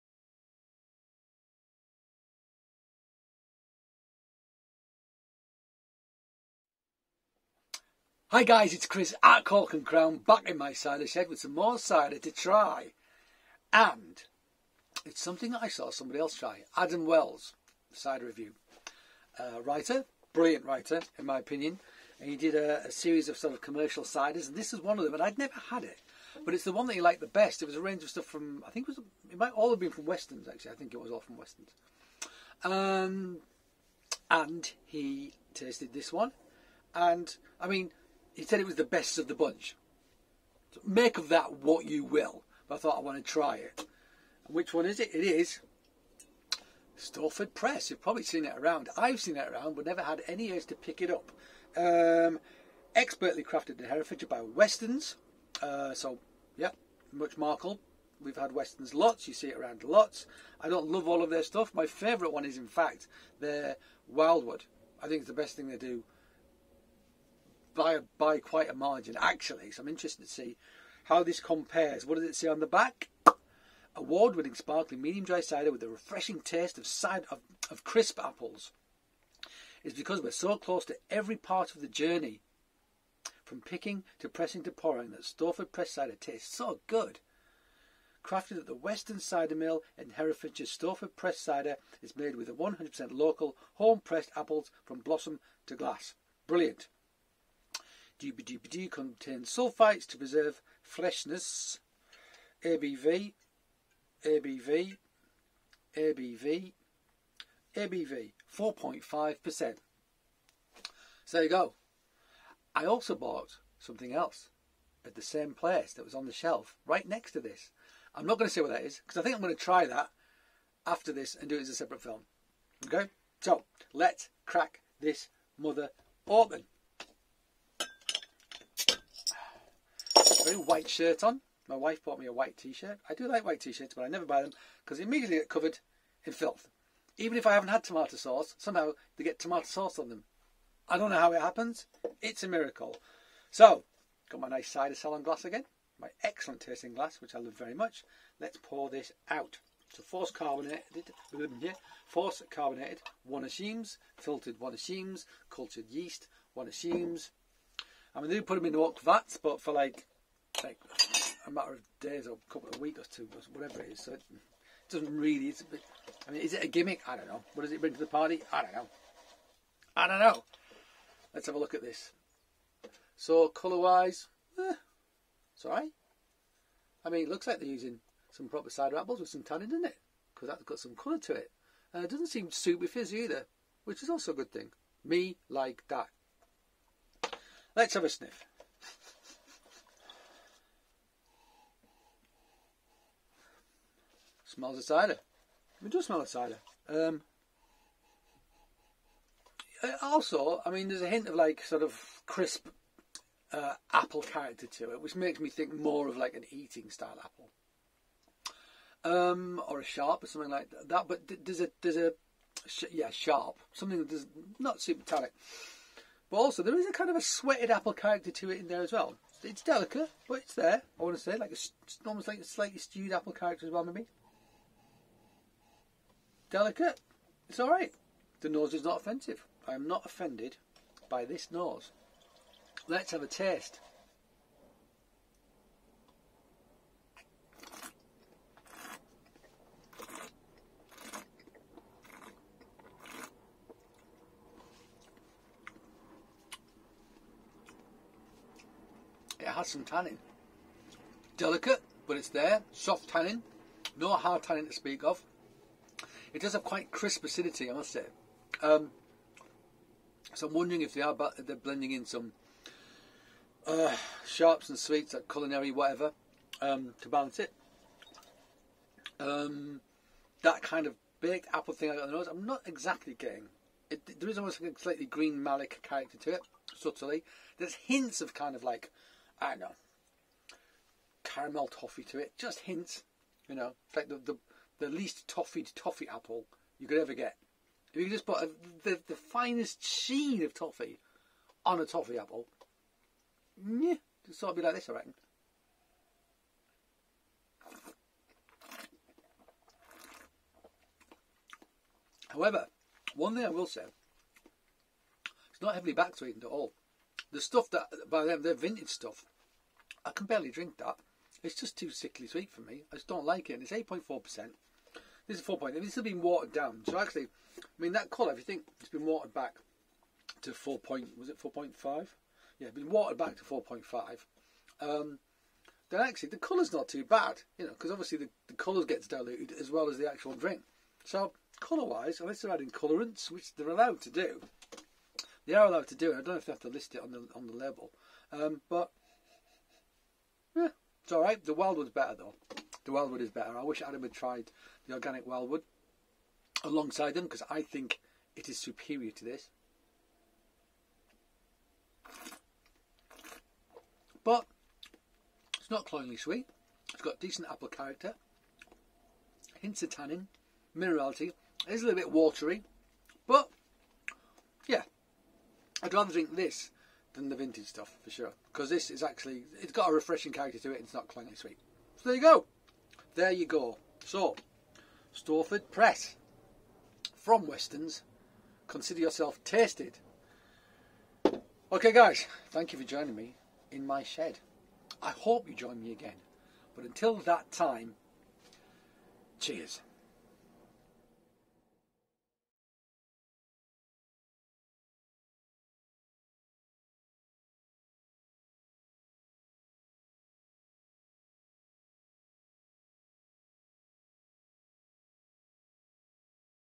#324 Weston's 'Stowford Press'
Chris tries a cider he's seen a lot but never tried before. Has he been missing out?